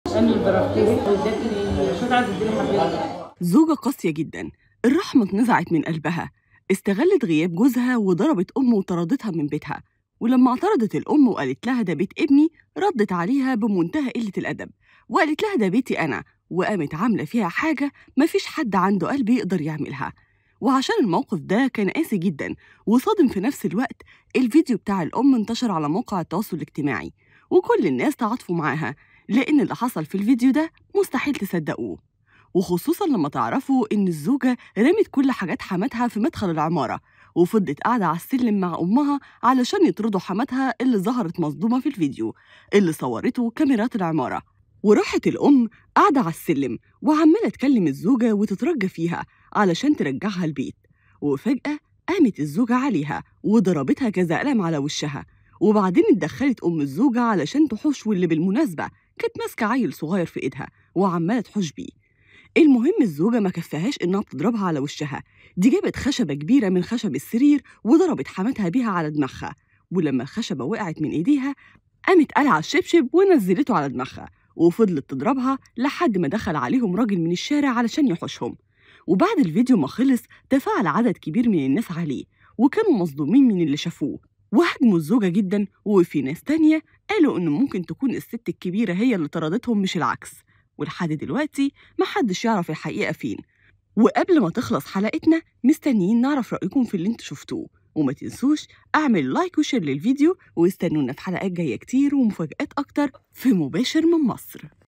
"استغلت غياب زوجها و طردت حماتها و ضربتها و رمتلها حاجاتها في بير السلم ترند ترندات اكسبلور"
"استغلت غياب زوجها و طردت حماتها و ضربتها و رمتلها حاجاتها في بير السلم